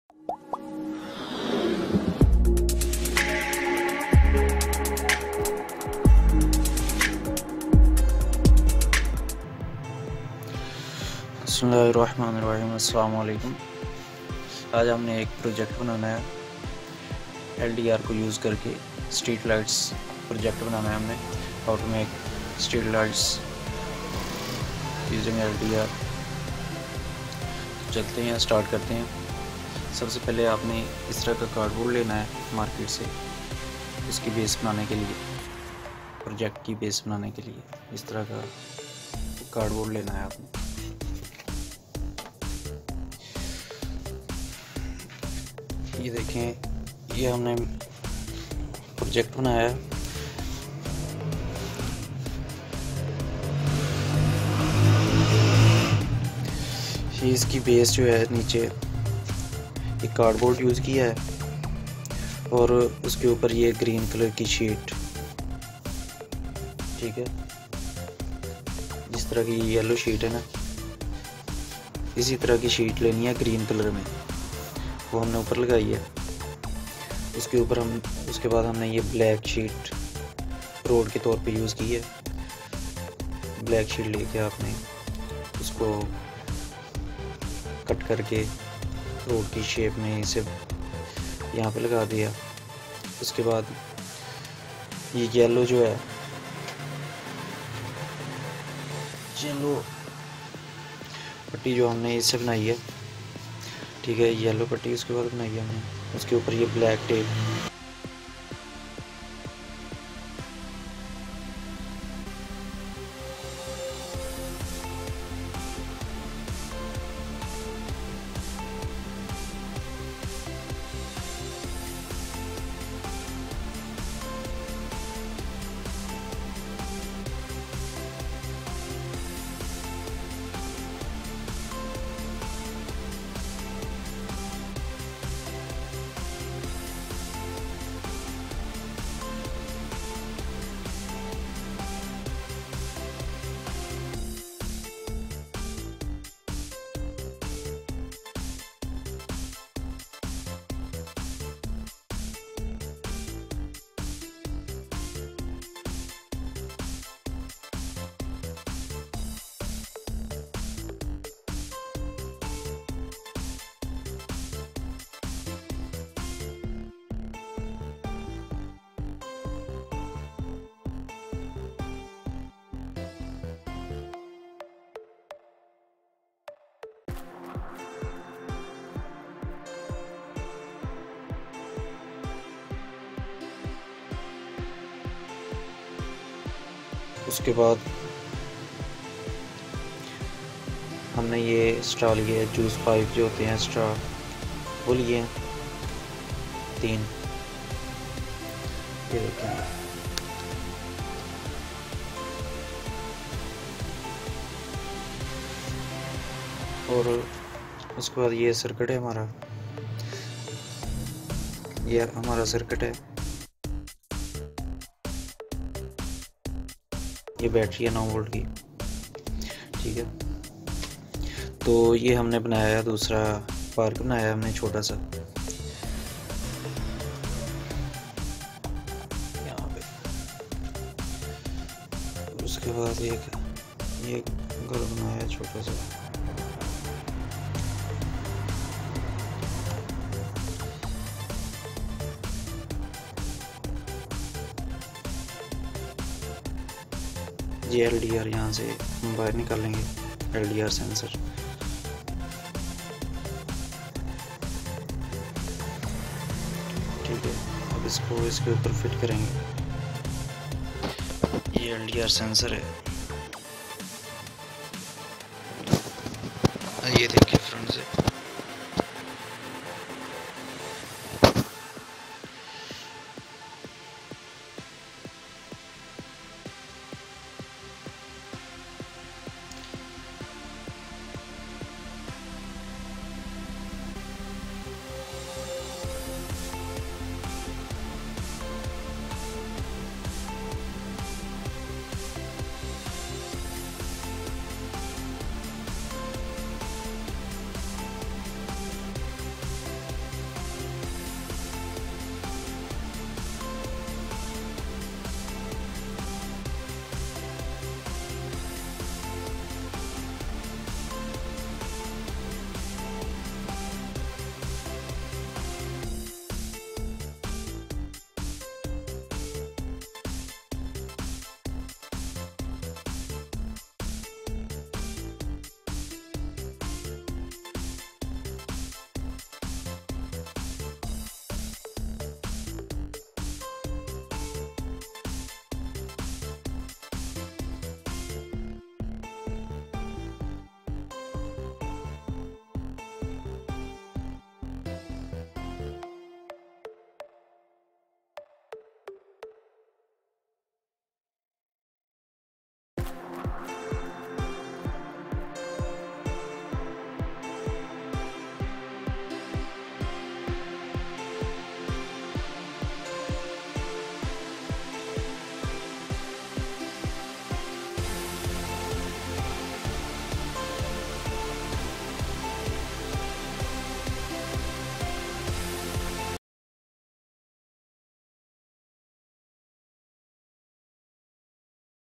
How to Make Auto Street Light Project Using LDR Sensor || Street Lights
How to Make Auto Street Light Project Using LDR Sensor
in this video i am going to show how to make auto street light project using LDR Sensor

Things Required:

1)  Cardboard
2)  Chart Paper (Green, Black, Yellow)
3)  Straw
4)  LED's

This channel is all about DIY, How To, Home Made, Great Ideas, simple, funny and entertainment.

Special thanks for YouTube Audio Music Library